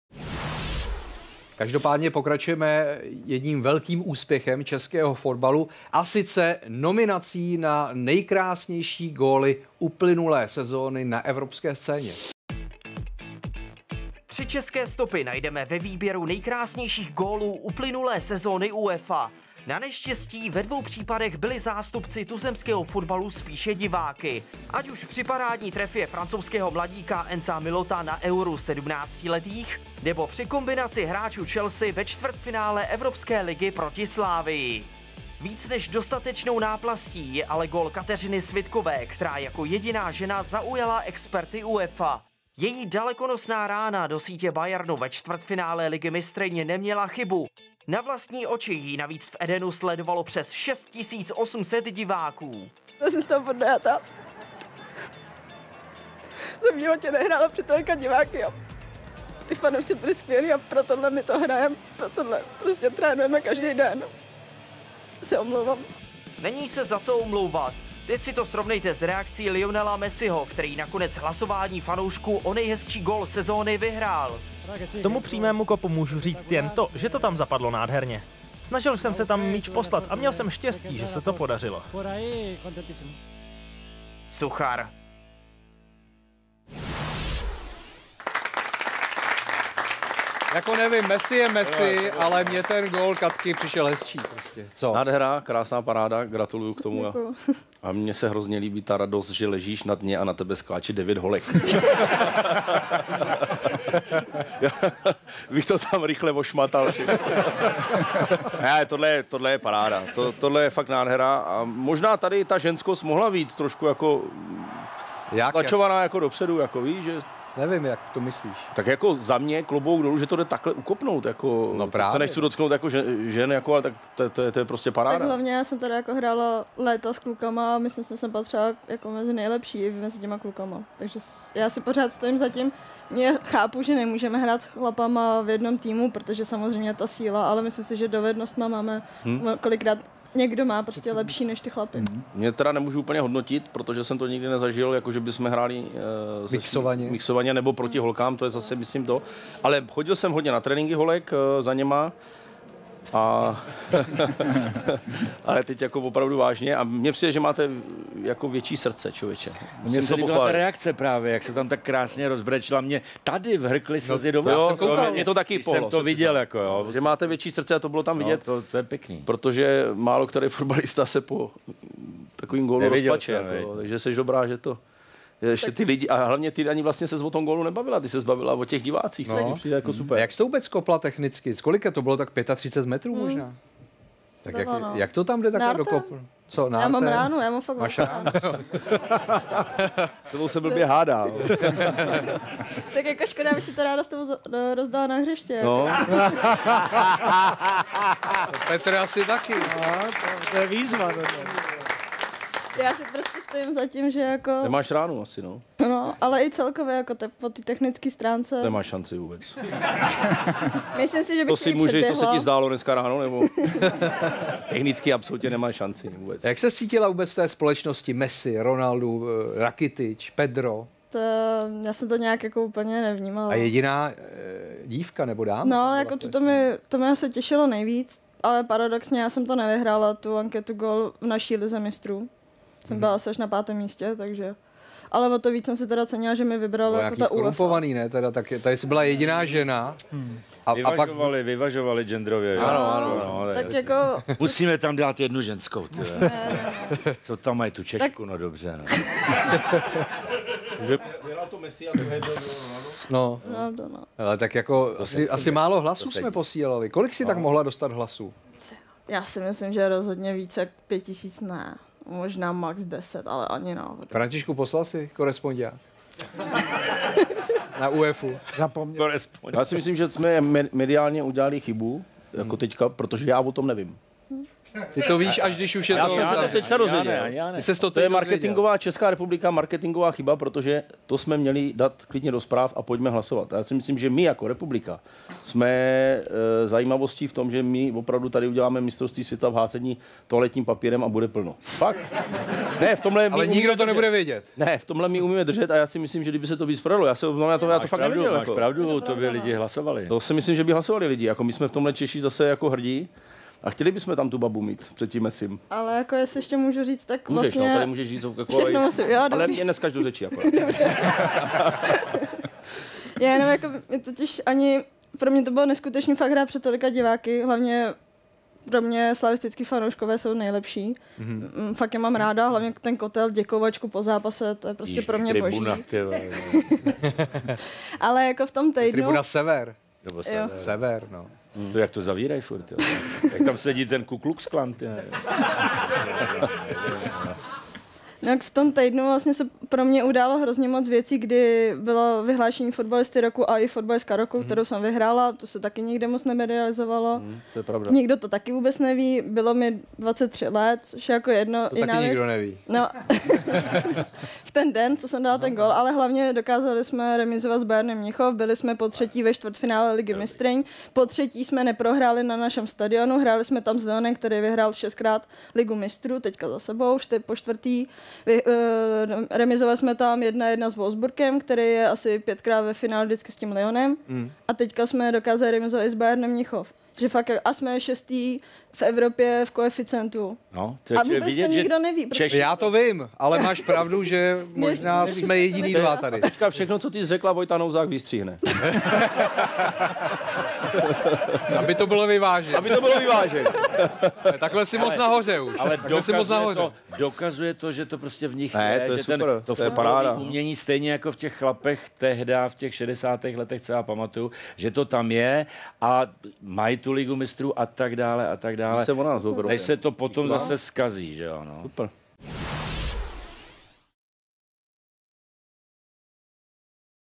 TIKI-TAKA: Kateřina Svitková proslavila český fotbal
Fotbalistka Slavie Kateřina Svitková zapsala obrovský úspěch pro český fotbal. Její gól proti Bayernu Mnichov byl totiž v nominaci na nejkrásnější evropský gól minulé sezony. A jak by podle Katky dopadl její souboj s Petrem Švancarou ve fotbalových dovednostech?